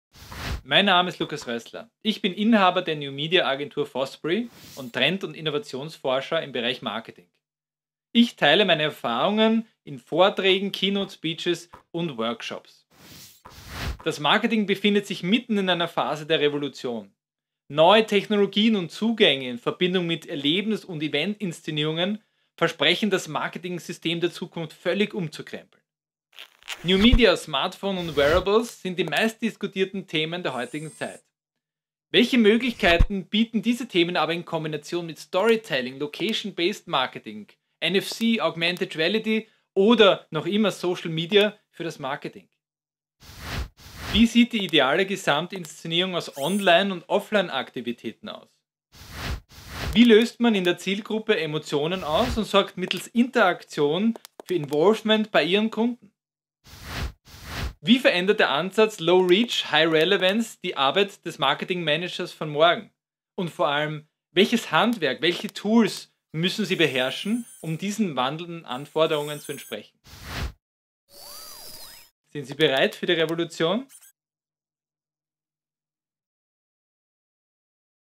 Speaker Lukas Rössler, CEO FOSBURY
Lukas Rössler ist Innovations- und Trendforscher im Bereich Marketing. Seine Erfahrung teilt er in Vorträgen, Keynote Speeches und In-House Workshops.

Das Marketing befindet sich mitten in einer Phase der Revolution: Neue Technologien und Zugänge in Verbindung mit innovativen Event- und Erlebnisinszenierungen versprechen das vorherrschende Marketing System völlig umzukrempeln.

Die Themen New Media, Smartphones und Wearables gehören zu den meistdiskutiertesten der heutigen Zeit. Welche Möglichkeiten bieten aber die Themen Storytelling, Location Based Marketing, Augmented Reality, Mobile Tagging, NFC und natürlich noch immer Social Media für das Marketing ?

Wie sieht die ideale Gesamtinszenierung aus „Online“ und „Offline“ Aktivitäten aus? Wie löst man in der Zielgruppe Emotionen aus und sorgt mittels Interaktion für Involvement beim Kunden? Wie verändert der Ansatz „Low Reach – High relevance“ die Arbeit der Marketing-Manager von morgen und vor allem: welches Handwerk müssen sie  beherrschen um den sich wandelnden Anforderungen zu entsprechen?

Sind sie bereit für die Revolution?  We know the fosbury trick!

- Mag. Lukas Rössler, MBA - Keynote Speaker, Referent und Trend-Workshop Leiter